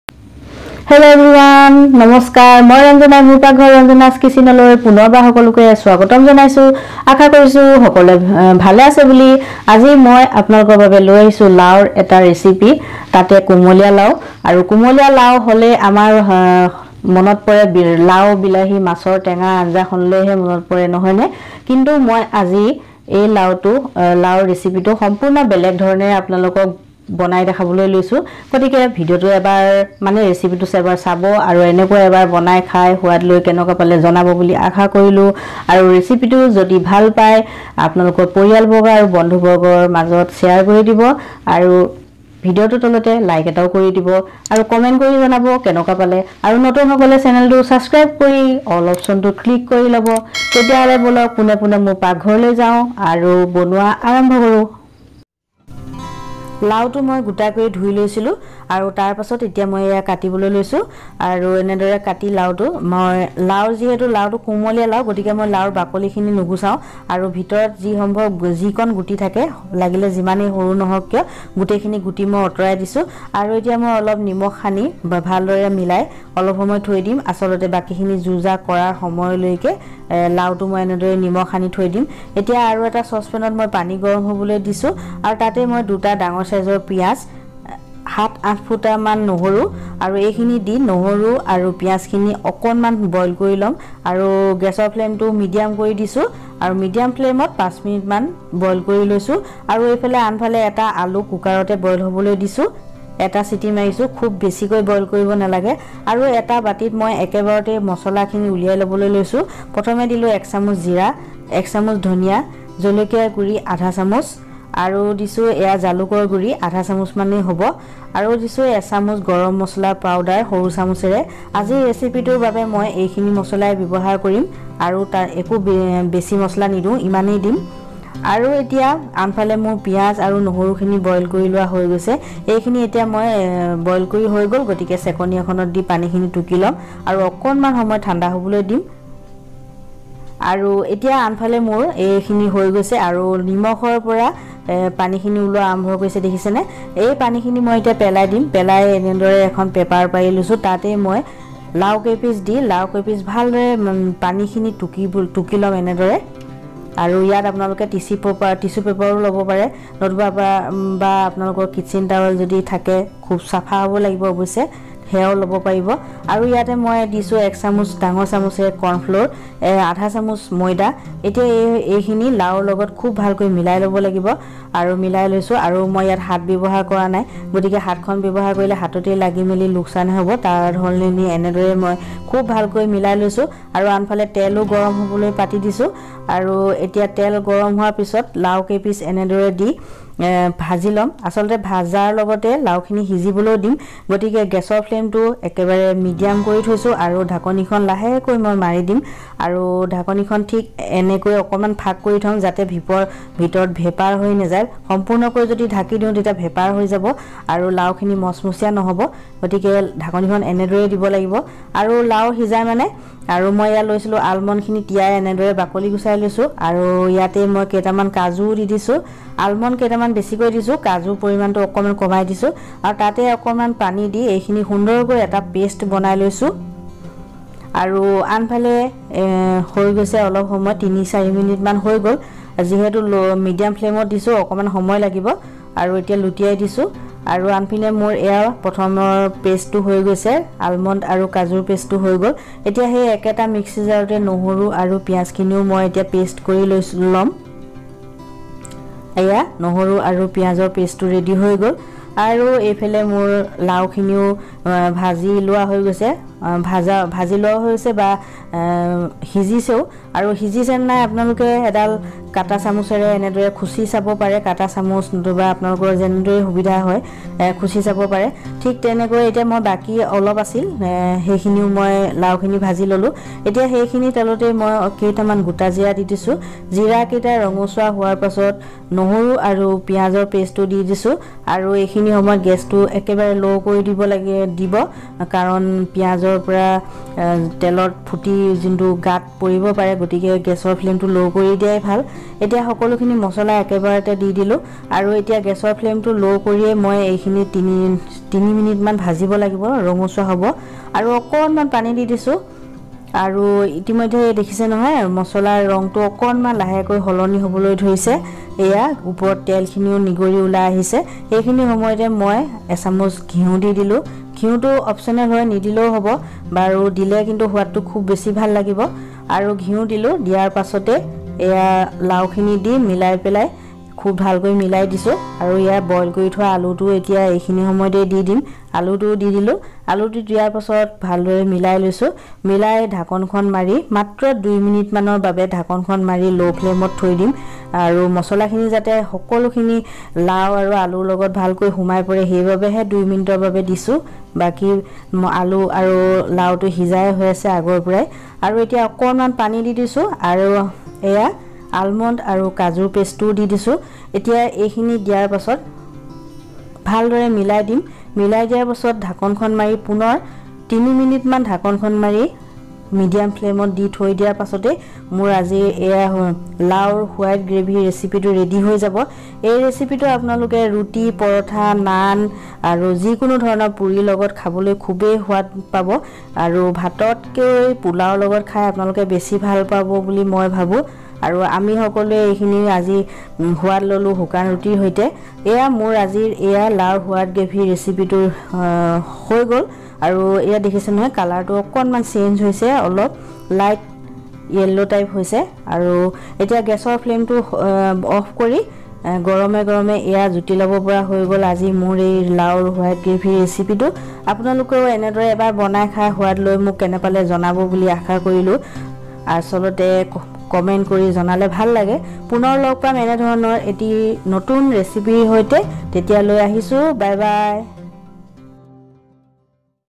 জাতি লাউ এনেকৈ খাইছেনে এবাৰ খালে বাৰে বাৰে খাবলৈ মন যাব/white gravy recipe/tasty white gourd recipe
Ingredients

white gourd
onion 2big size
garlic
almond
causunuts
cumin powder
coriander powder
Red chili powder
garam masala
ghee
refined oil

ৰেচিপিটো কেনেকুৱা পালে জনাব বুলি আশা কৰিলো লাইক কমেণ্ট চেয়াৰ আৰু চাচক্ৰাইব কৰিব বুলি আশা কৰিলো ধন্যবাদ 🙏